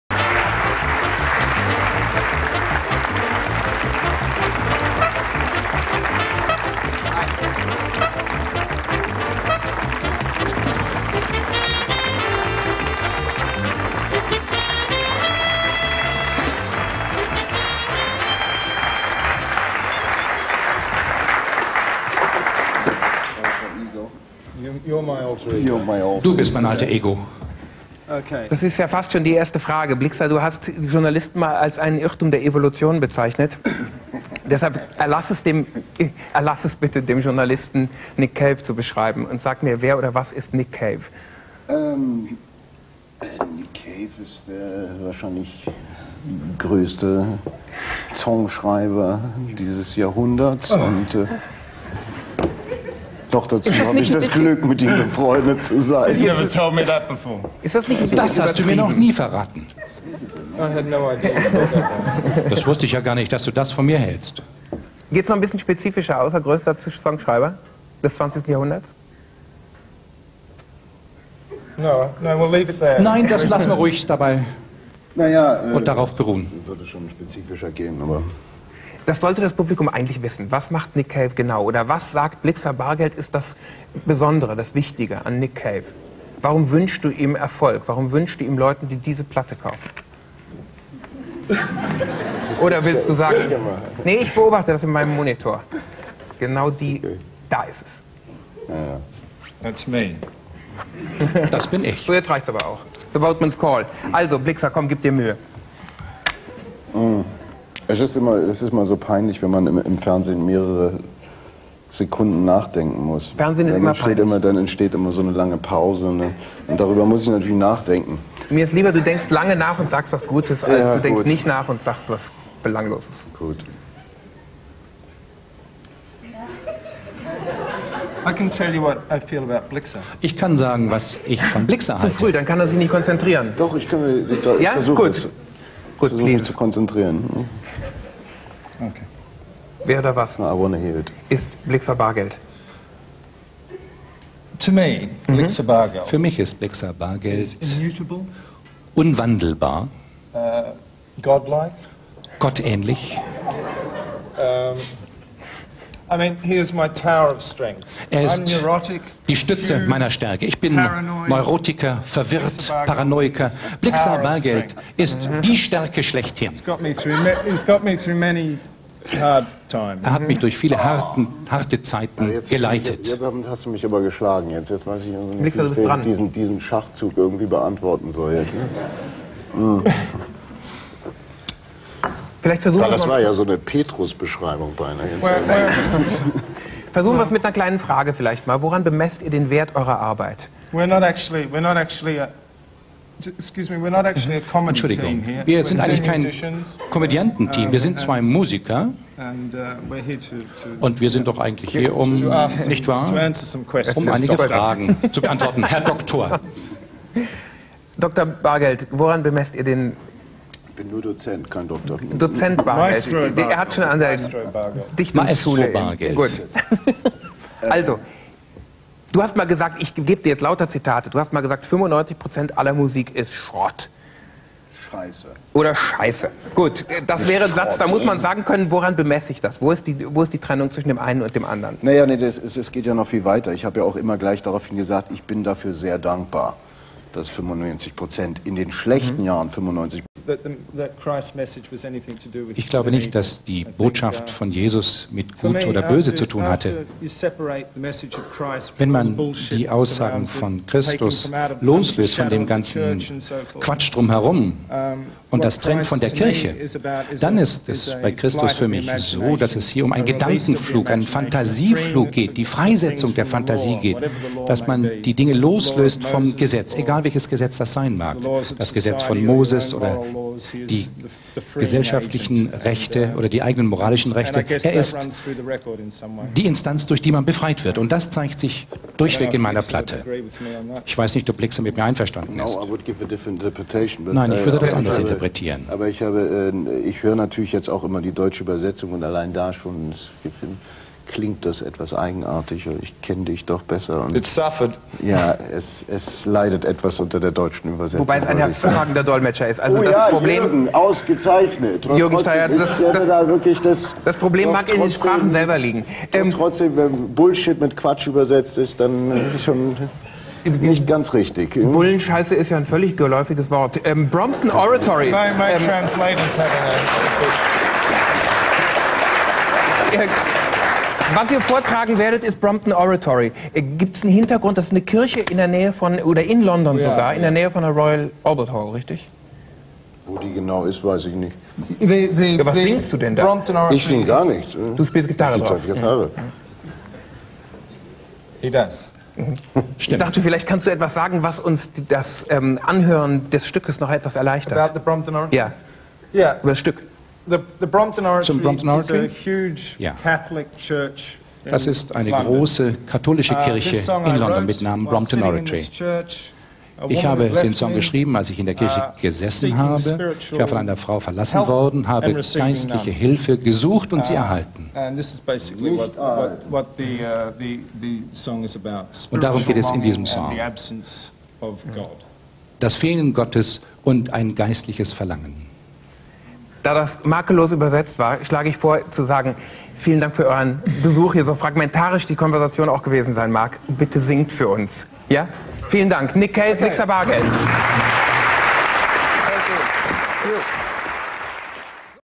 From the Archive: Nick Cave and Blixa Bargeld
It all gets a bit confusing as Nick Cave and Bad Seeds bandmate have an interview on German TV.